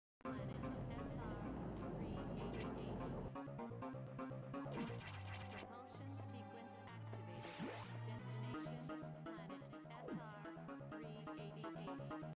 Leaving Operations Deck Door Transition (TAS)
Not boosting is usually faster if jump is high enough because boost is almost impossible to optimize this well.